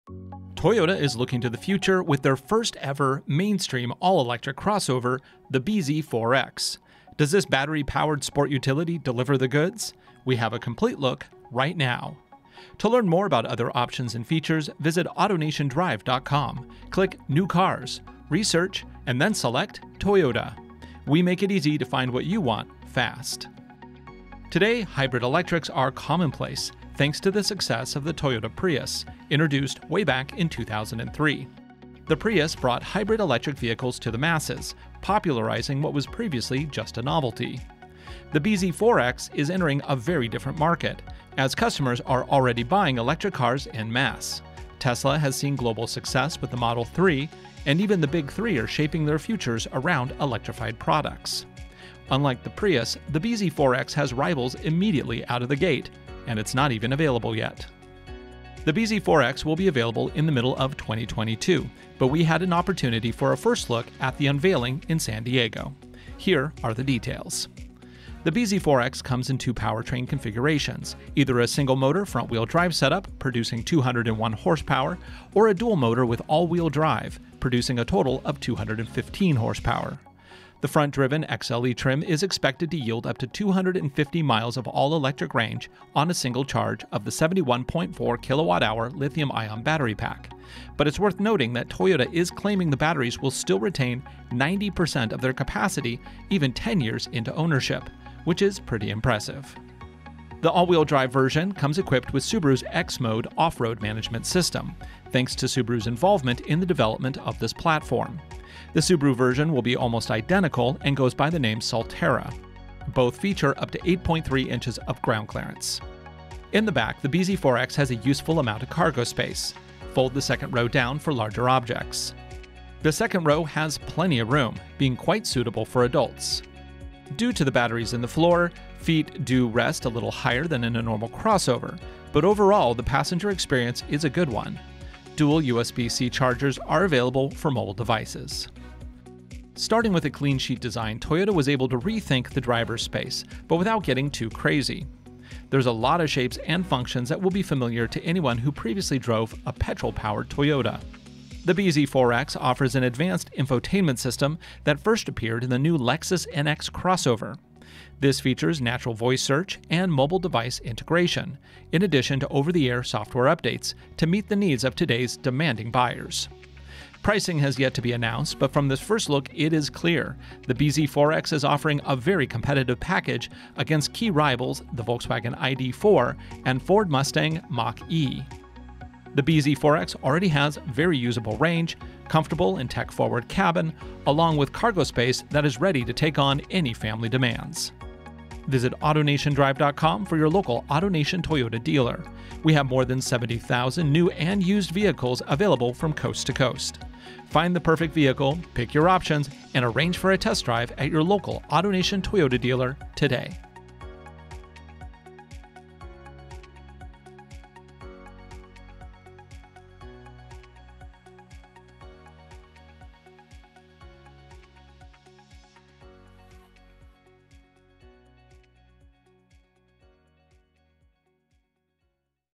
Toyota BZ4x Preview AutoNation
Toyota is looking to the future with their first ever mainstream all-electric crossover: the Bz4X. Does this battery-powered sport utility deliver the goods? We have a complete look, right now.

Dealership locations, automotive service and repairs, coast to coast!